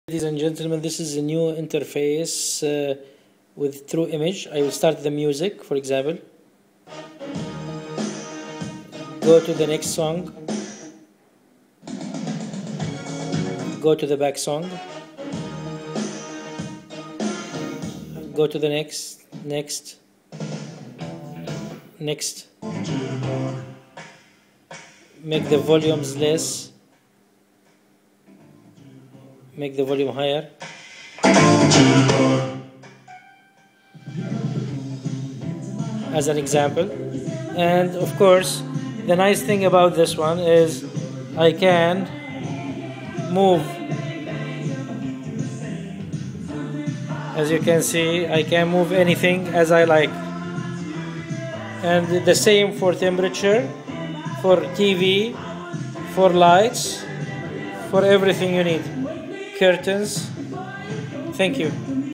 Simple Image Interface App2 , Smart-BUS G4 Home Automation
Smart-BUS G4 Home Automation
Simple Image Interface App , Smart-BUS G4 Home Automation Smart-BUS G4 Home Automation.
Simply Take a Picture of your Room, Add the Items icons that you need to Control
Put the addresses, and Simply Control